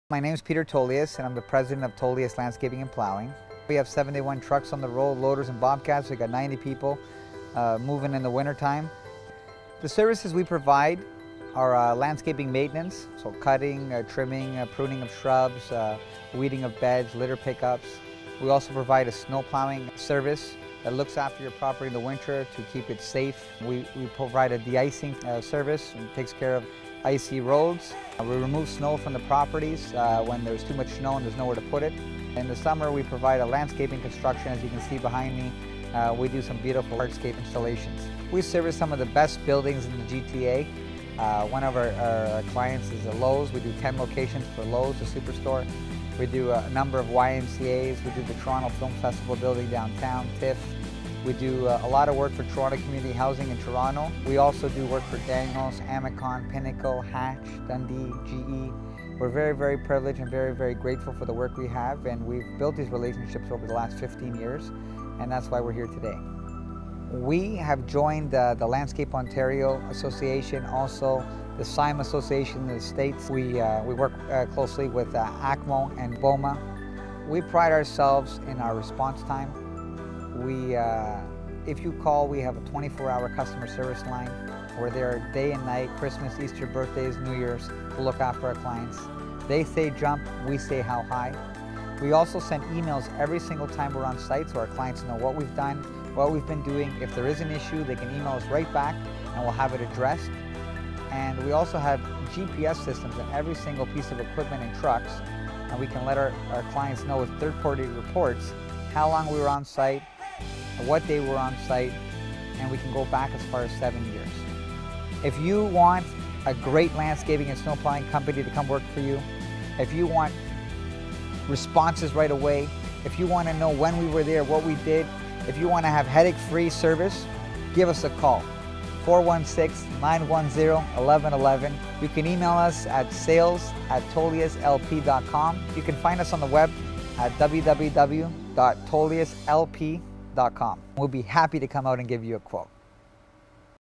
Tolias Landscape and Plowing Promo Video
We provide landscaping and plowing services for commercial properties across the greater Toronto area.

Thanks for stopping by and have a wonderful day!